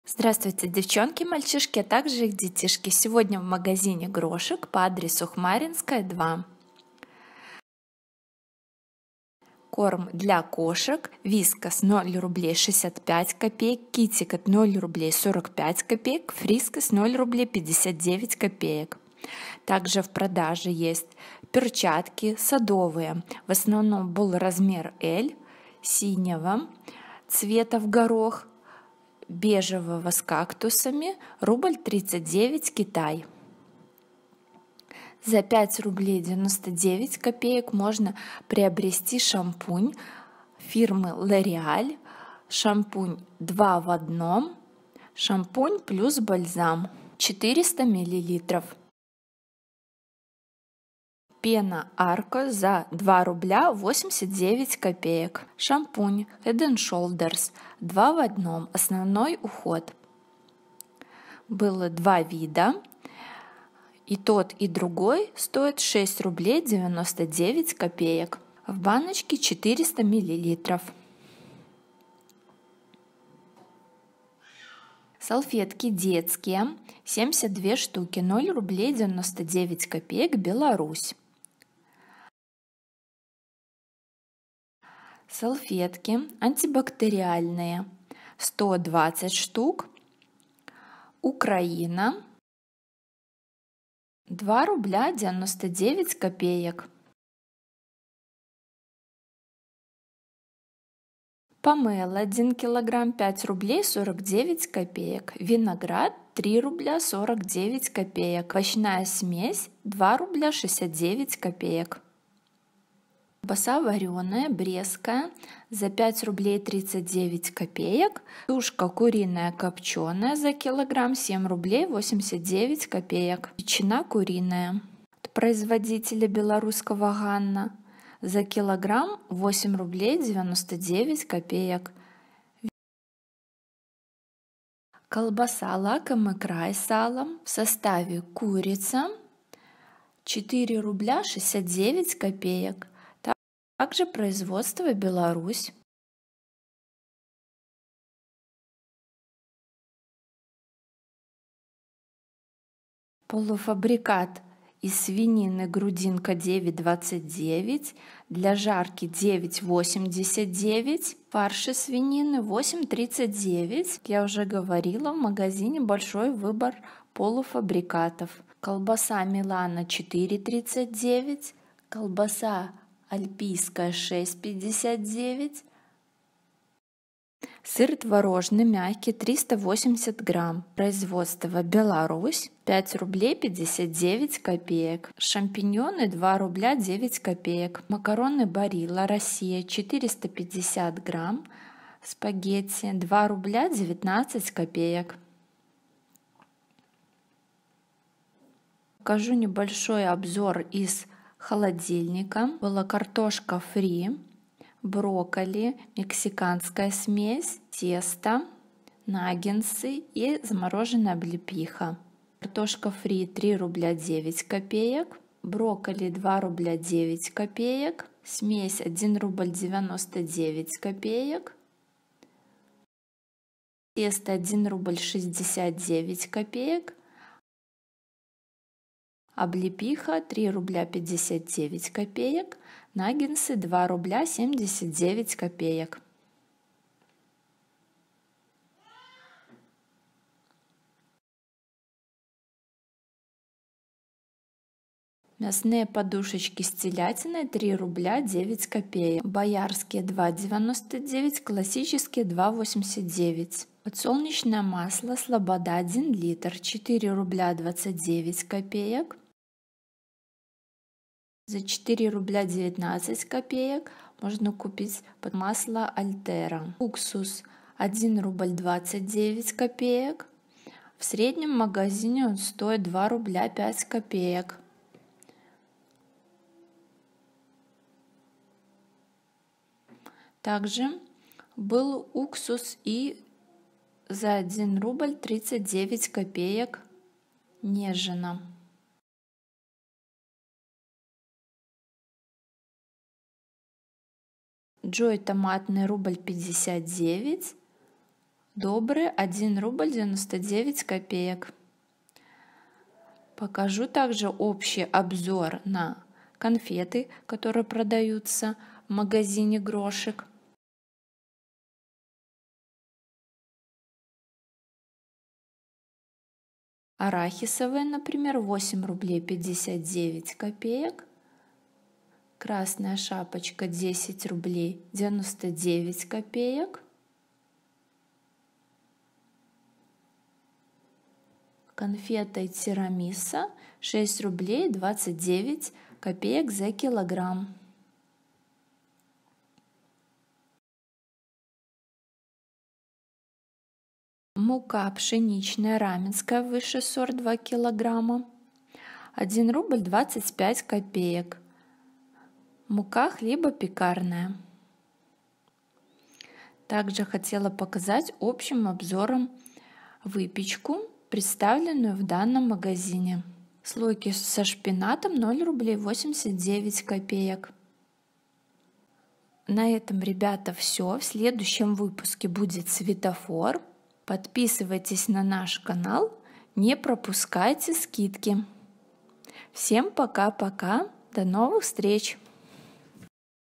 ГРОШЫК💚💙 Минск Беларусь🧡 🛒
Обзор товаров из магазина Грошык, расположенного по адресу г. Минск, ул. Хмаринская, 2/1